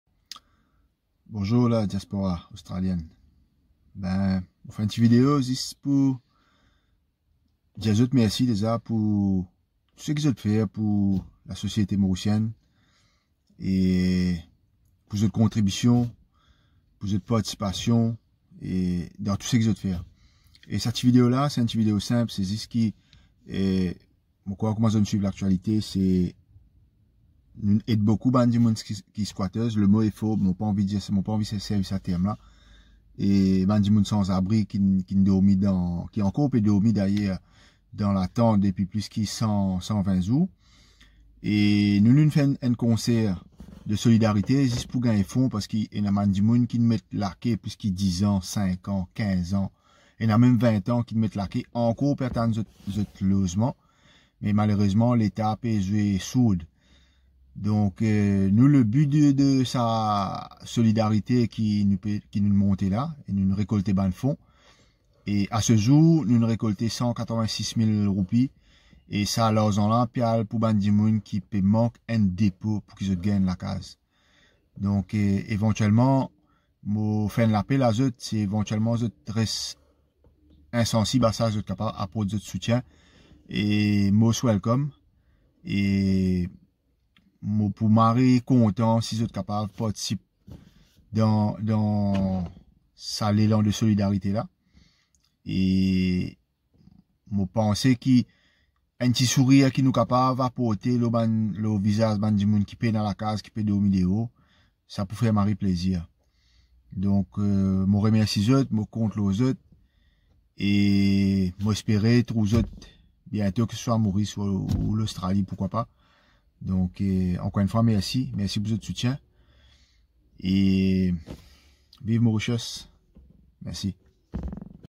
Stephane Buckland talks to Mauritian Diaspora in Australia
Stephane Buckland requests the help of the Mauritian Diaspora in Australia to support homeless people in Mauritius